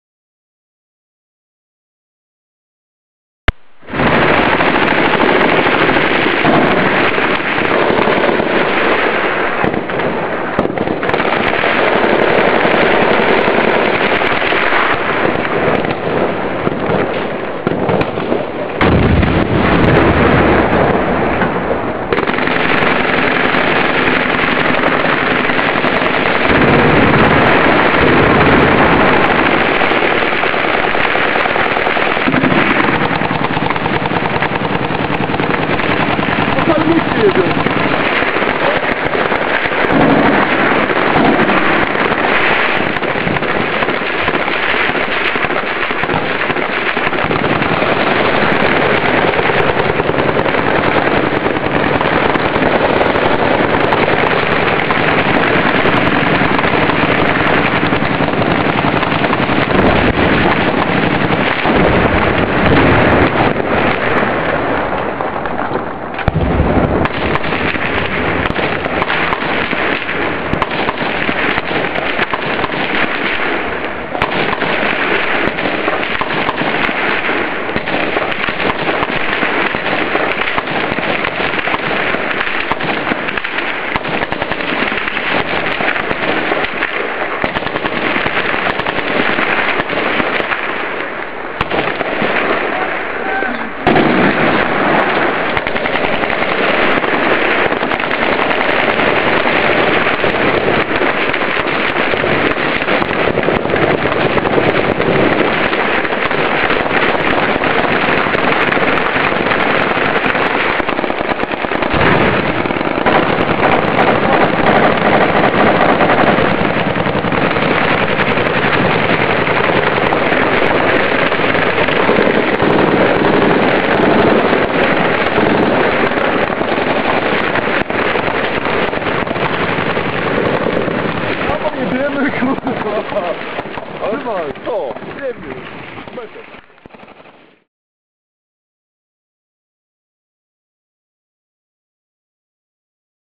Obrona City Hall w Karbali. Największa bitwa polskich żołnierzy od II Wojny Światowej
Wiosną 2004 r. zwiadowcy z międzyrzeckiej brygady, dowodzeni przez kapitana Grzegorza Kaliciaka, przez trzy dni bronili ratusza (City Hall) w Karbali w Iraku przed atakami islamskich bojówkarzy. To była największa bitwa stoczona przez polskich żołnierzy po drugiej wojnie światowej. Brało w niej udział 40 międzyrzeczan wspieranych przez pluton Bułgarów.